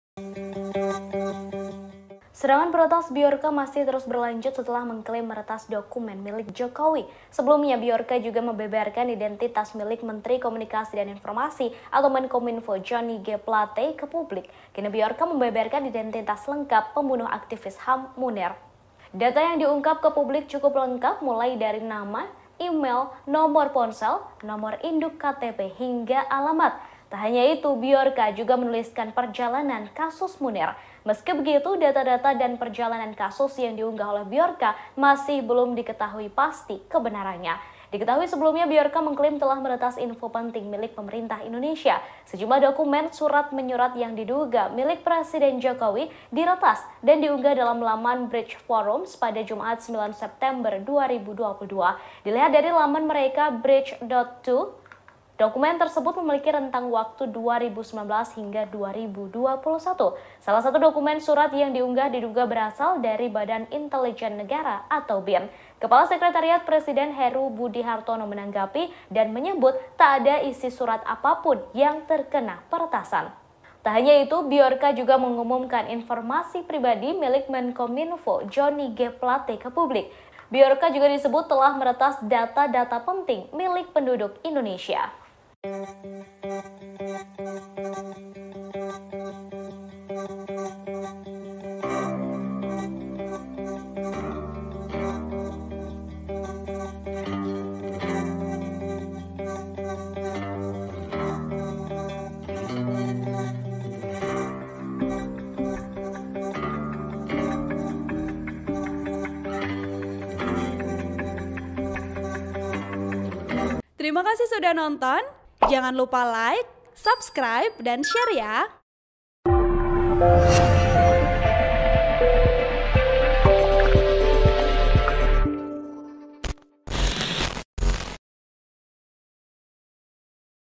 Peretas Bjorka Beberkan Identitas Pembunuhan Munir
AN TRIBUNAMBON.COM Peretas Bjorka Beberkan Identitas Pembunuhan Munir